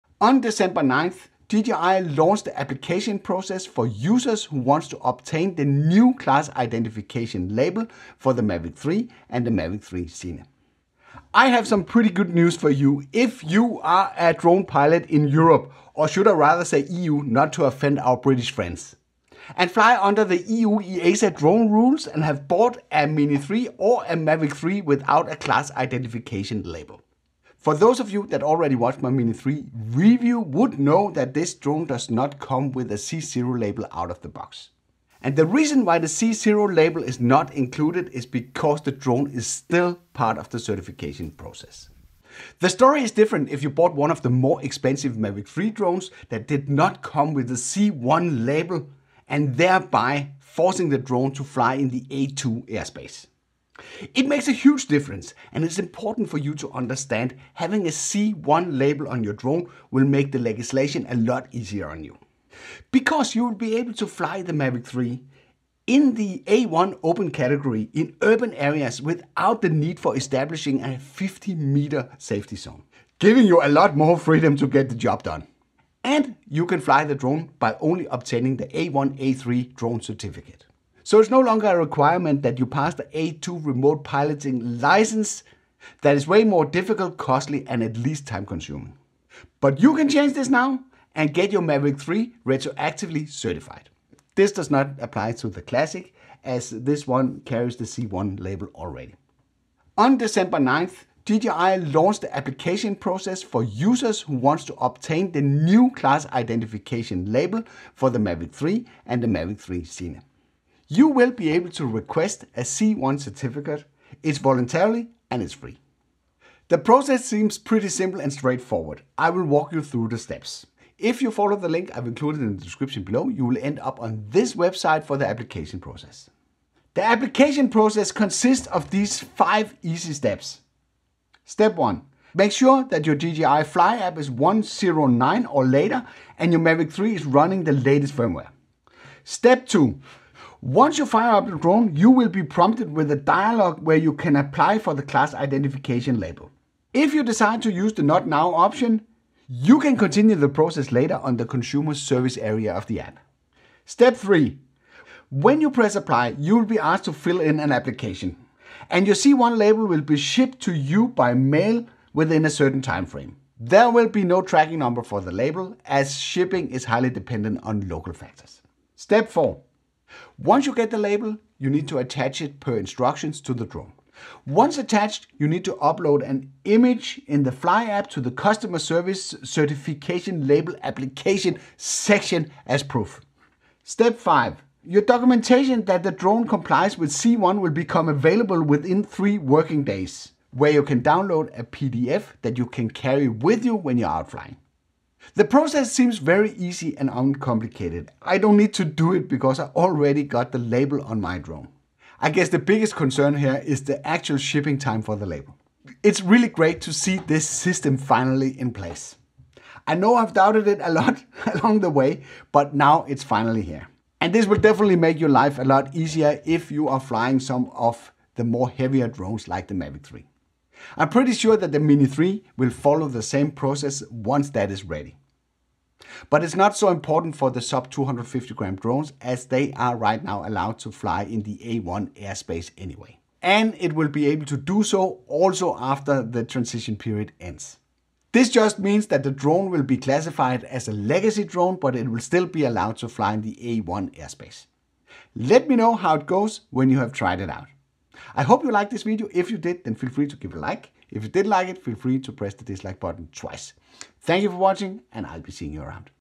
How to get a C1 Class Identification label for DJI Mavic 3? (+ DJI Mini 3 C0 Update)
On December 9th DJI launched the application process for customers who wish to obtain the new C1 class identification label for the Mavic 3, and Mavic 3 Cine drones. Users will be able to request the C1 certificate voluntarily - and it’s FREE tough this link: APPLY

✅ If you already made up your mind, you can grab one

-- IMPORTANT --